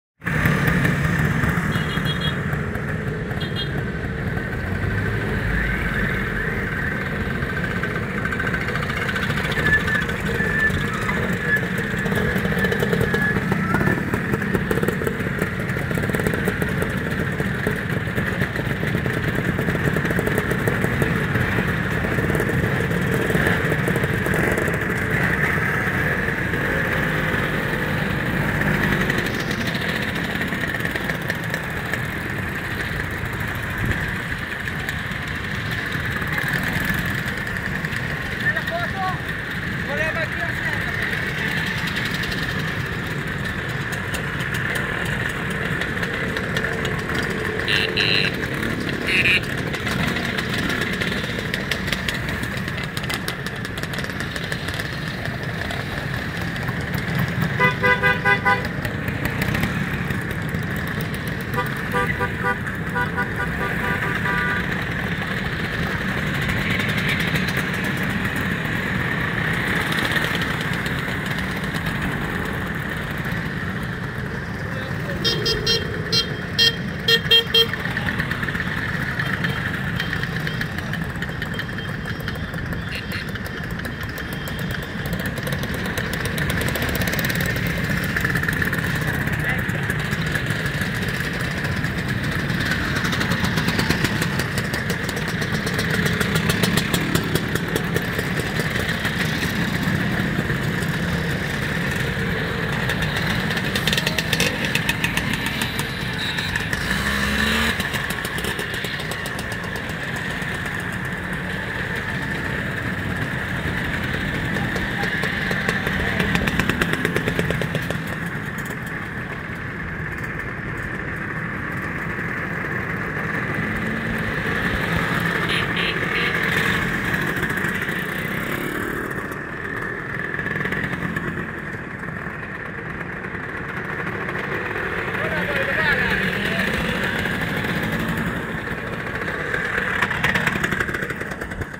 11°Raduno VespaClub Città di Piombino, Memorial Aviano Contri
La partenza dalla piazza di Cittadella